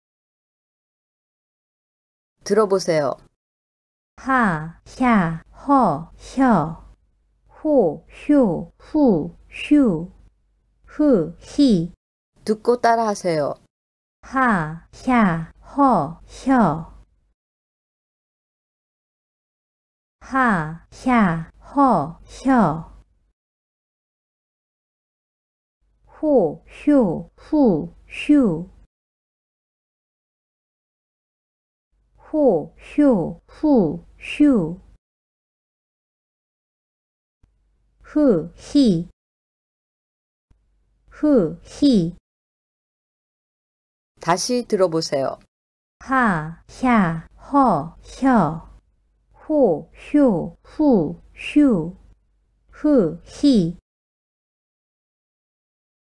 Hangeul 하 햐 허 혀 호 효 후 휴 흐 히
This is a practice video for Unit 2, My Korean 1. This book is downloadable free of charge at